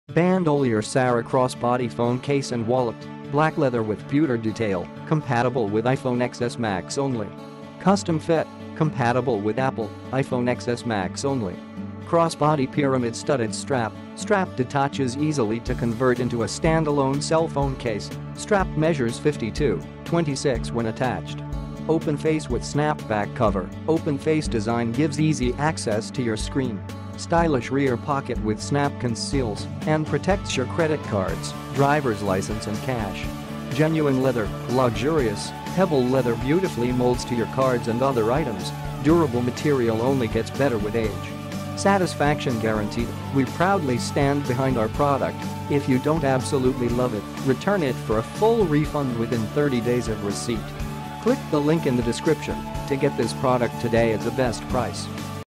Bandolier Sarah Crossbody Phone Case and Wallet
Click here for the best price:

 Bandolier Sarah Crossbody Phone Case and Wallet - Black Leather With Pewter Detail - Compatible With iPhone XS Max Only

 CUSTOM FIT - Compatible with Apple iPhone XS Max only  CROSSBODY PYRAMID STUDDED STRAP - Strap detaches easily to convert into a stand-alone cell phone case; Strap measures 52"" (26” when attached)  OPEN-FACE WITH SNAPBACK COVER - Open-face design gives easy access to your screen; Stylish rear pocket with snap conceals and protects your credit cards, driver's license and cash  GENUINE LEATHER - Luxurious, pebble leather beautifully molds to your cards and other items; Durable material only gets better with age  SATISFACTION GUARANTEED - We proudly stand behind our product; If you don’t absolutely love it, return it for a full refund within 30 days of receipt "